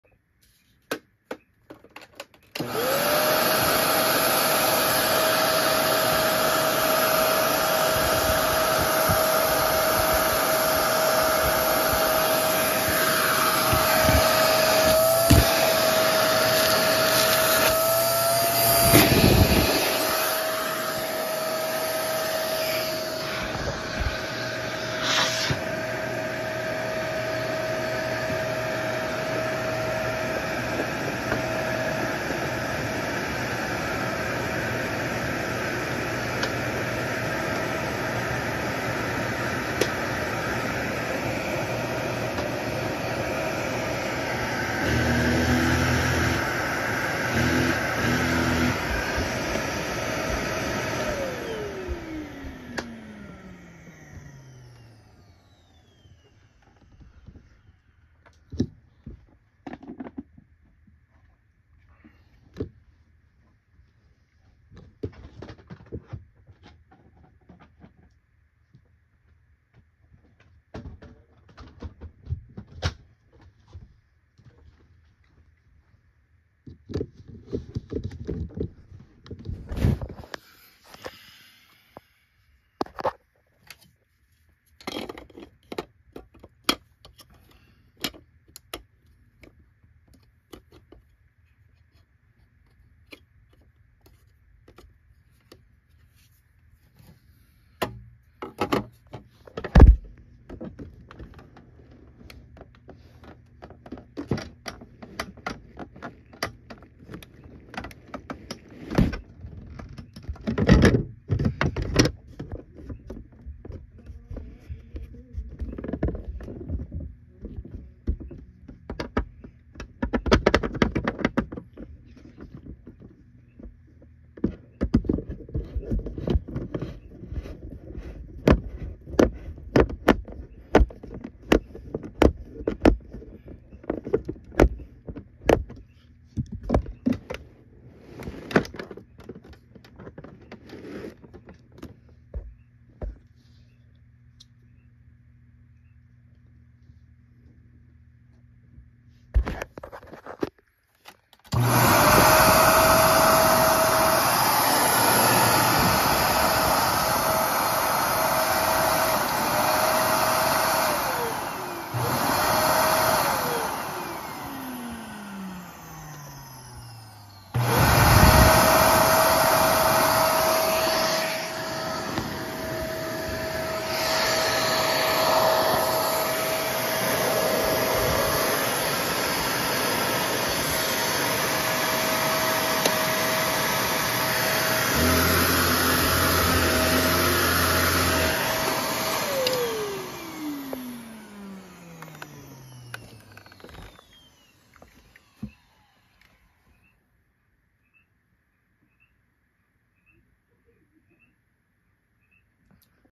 Trying to fix my Savvy.
The Sensor and headlight were always like this when I got it. How can I fix it?

Manufactured by Maytag in Mexico during Feb 2006.